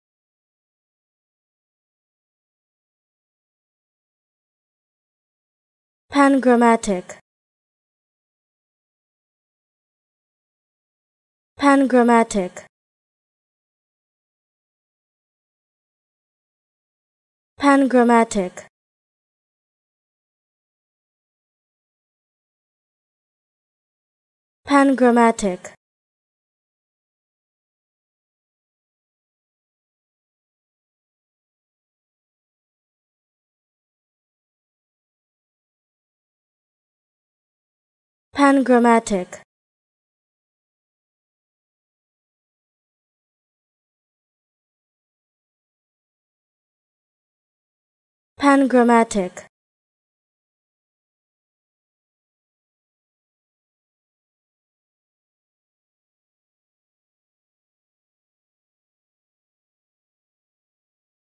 How To Say Pangrammatic
Pronunciation of Pangrammatic: Learn how to pronounce the word Pangrammatic.